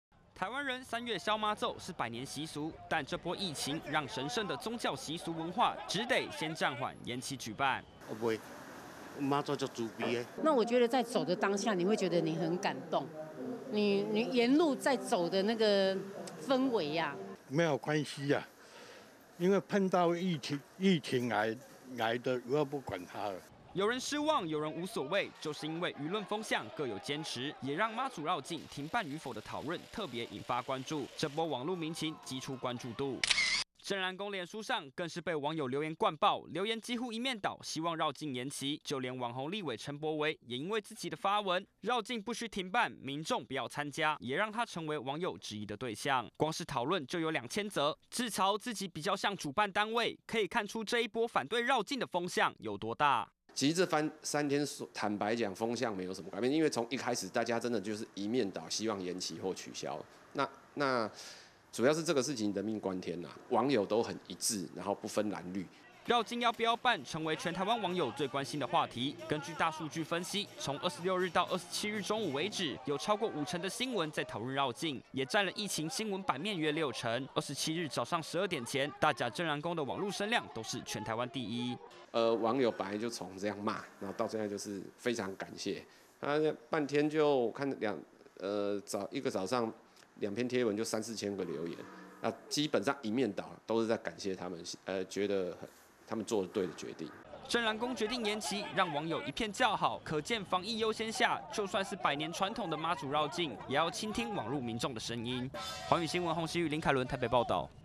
''三月瘋媽祖''全喊卡 網友風向促遶境延期｜寰宇新聞 20200227
【寰宇新聞】

疫情升溫，全台血庫拉警報，再加上，指揮中心也規定，從中港澳日韓等地，返台的國人暫緩捐血28天，讓血庫存量雪上加霜，現在台北和高雄，每一種血型都很缺，而且已經低於7天的血液安全庫存量